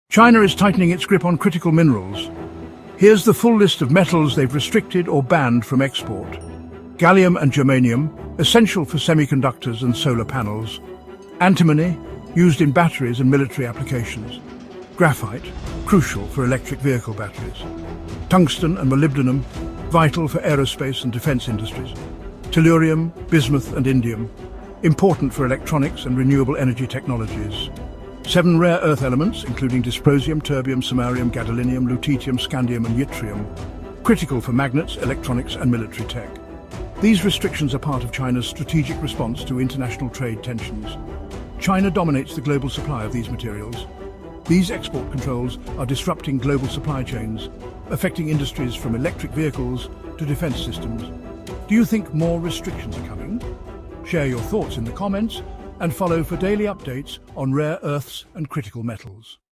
China’s Critical Metals Export Ban - FULL LIST
The Metals China Has Banned from Export — Full List

China is tightening its grip on critical minerals. Here’s the full list of metals they’ve restricted or banned from export:

 • Gallium and Germanium — essential for semiconductors and solar panels.
 • Antimony — used in batteries and military applications.
 • Graphite — crucial for electric vehicle batteries.
 • Tungsten and Molybdenum — vital for aerospace and defense industries.
 • Tellurium, Bismuth, and Indium — important for electronics and renewable energy technologies.
 • Seven rare earth elements, including Dysprosium, Terbium, Samarium, Gadolinium, Lutetium, Scandium, and Yttrium — critical for magnets, electronics, and military tech.

These restrictions are part of China’s strategic response to international trade tensions and tariffs.

China dominates the global supply of these materials. These export controls are disrupting global supply chains, affecting industries from electric vehicles to defense systems.